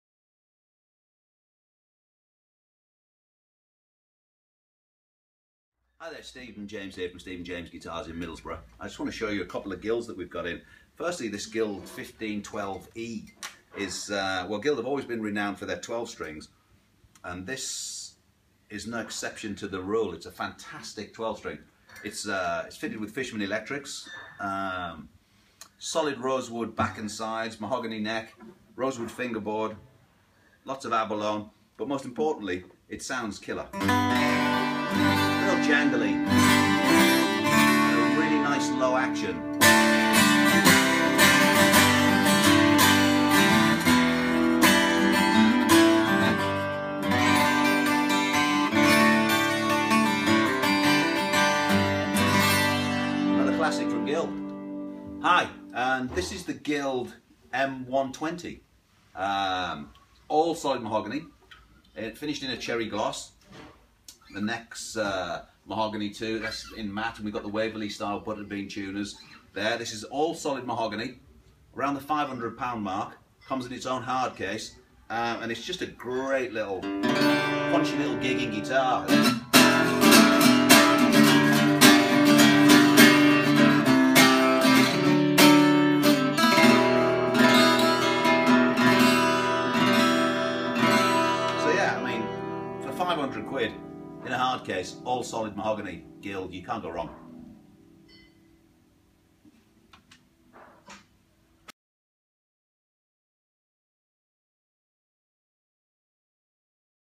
Guild Acoustics
Steven James Demos Two Guild Acoustic Guitars the Guild M-120 - Cherry Red and The Guild F-1512E Jumbo 12-String Electro - Natural